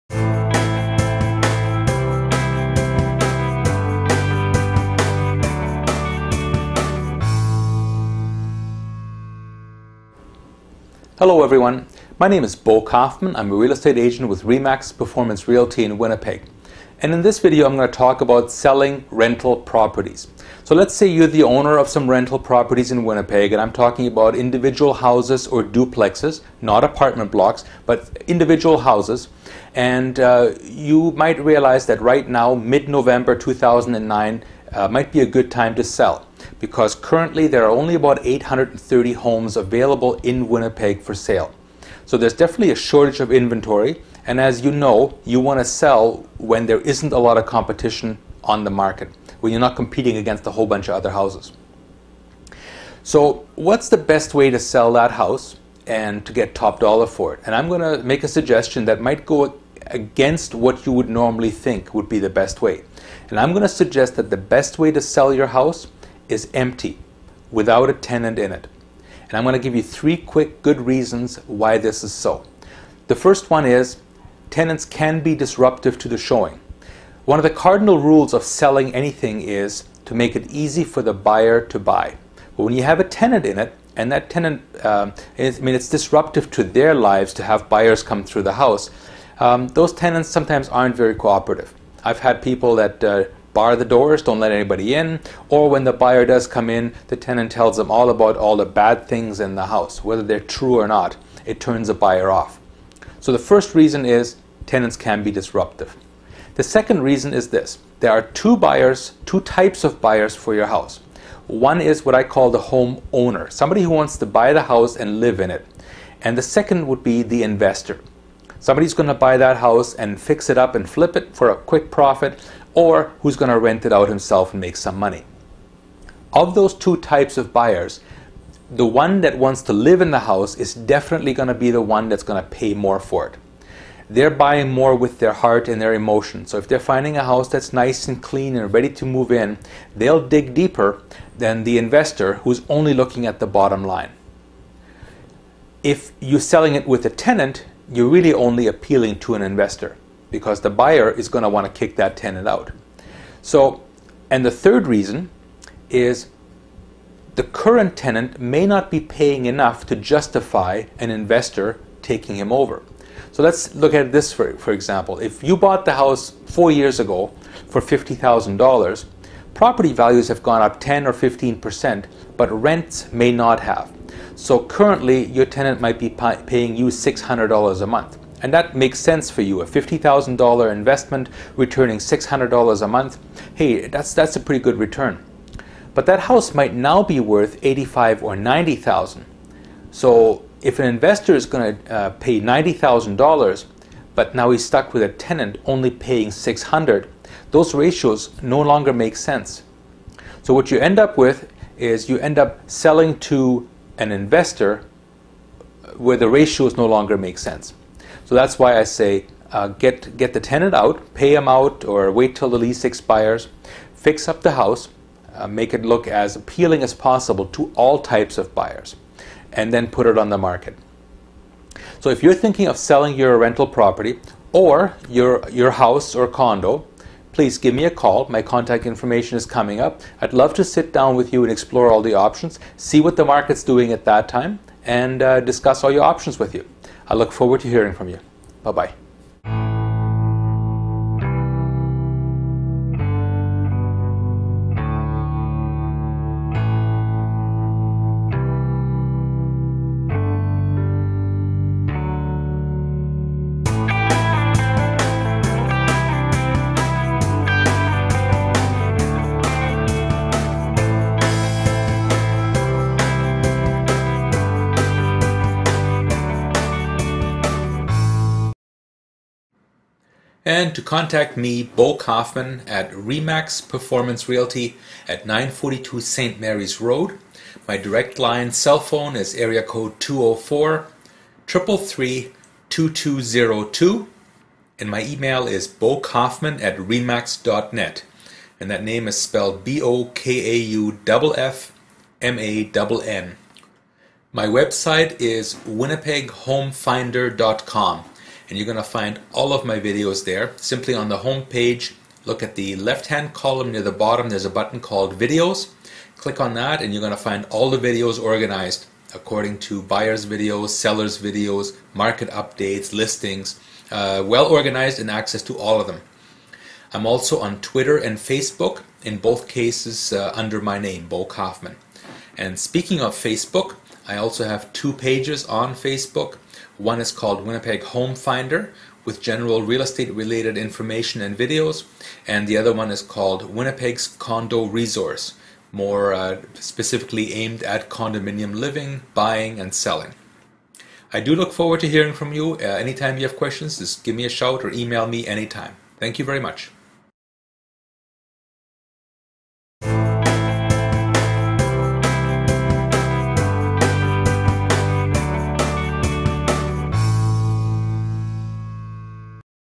How to sell your rental property in Winnipeg, selling a hou
Owners of rental properties may decide that NOW is a good time to sell. This video explains the best way to sell rental properties and get top dollar